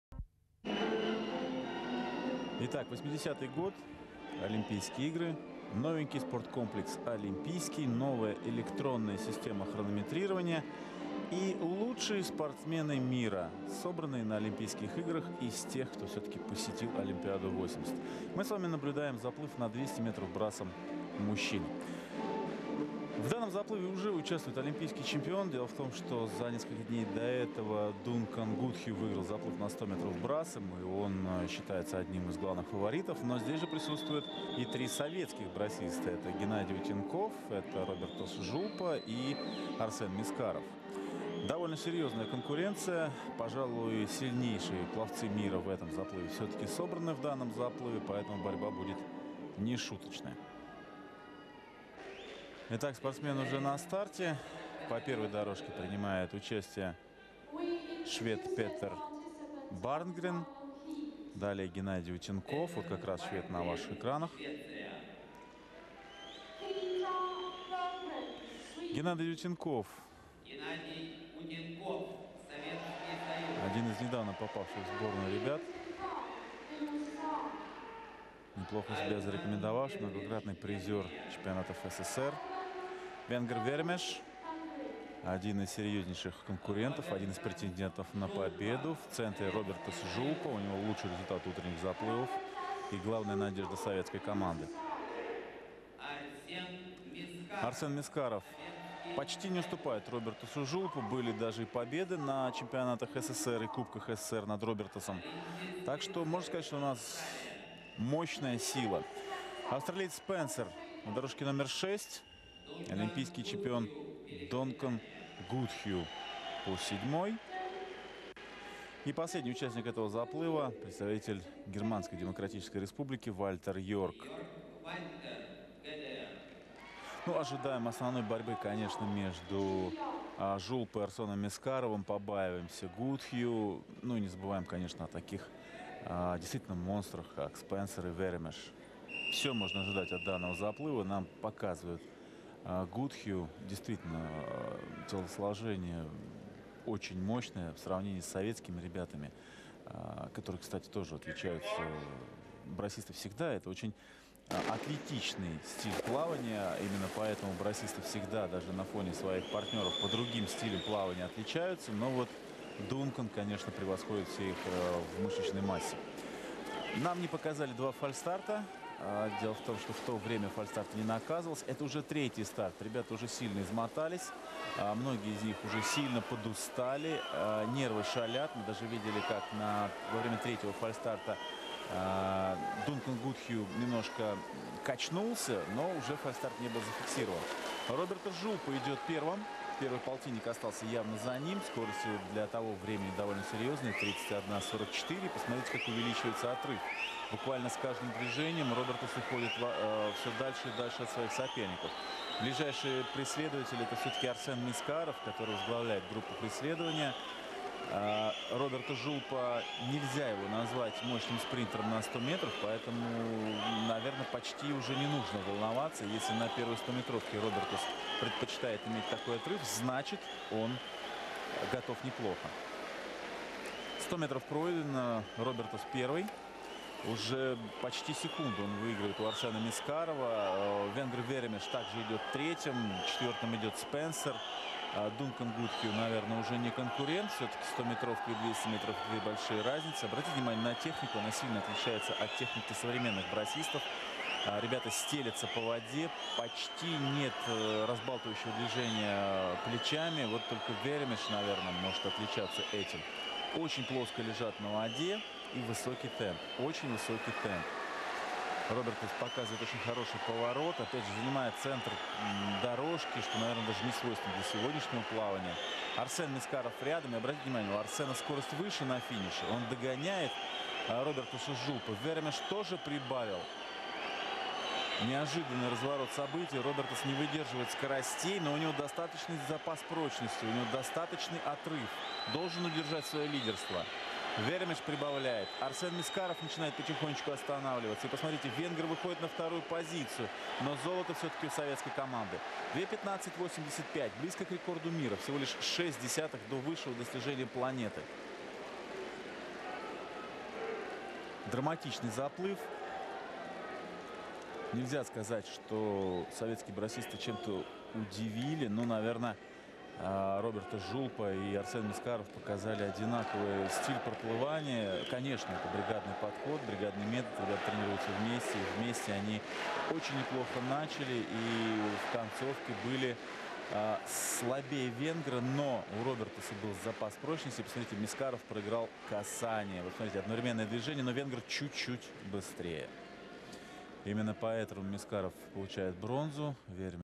1980 Olympic Men's 200m Breaststroke final - Robertas Zhulpa
Финал Олимпийских игр 80 200 брасс
Olympic games 1980 final 200 meter Breaststroke